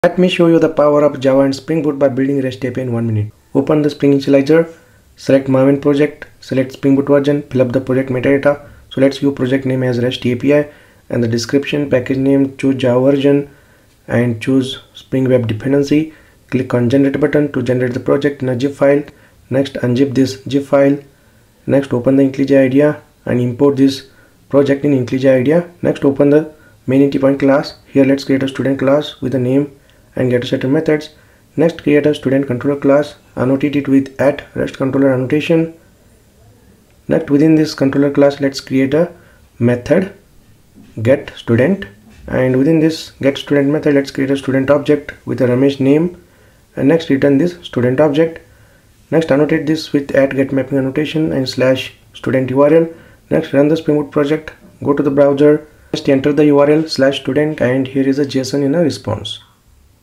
Build Java REST API using Spring Boot in 1 Minute | Power of Java and Spring Boot
Watch how easy it is to create a fully functional REST API in Spring Boot in under a minute! Leverage Spring Boot's auto-configuration, embedded server, and simple annotations like @RestController and @GetMapping to build powerful APIs quickly. The simplicity of Java and Spring Boot makes API development fast and efficient!